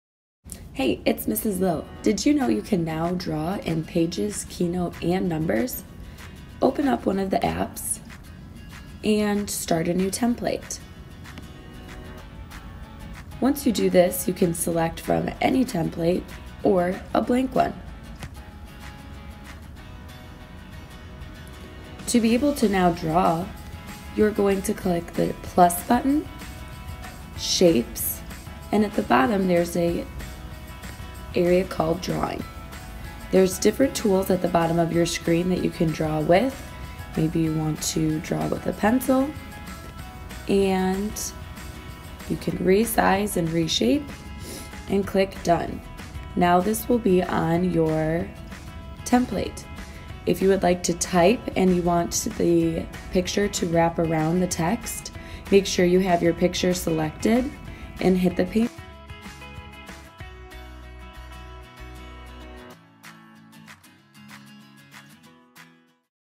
Draw in Pages, Keynote & Numbers!
Watch this video to learn how to draw in Pages, Keynote and Numbers on the iPad.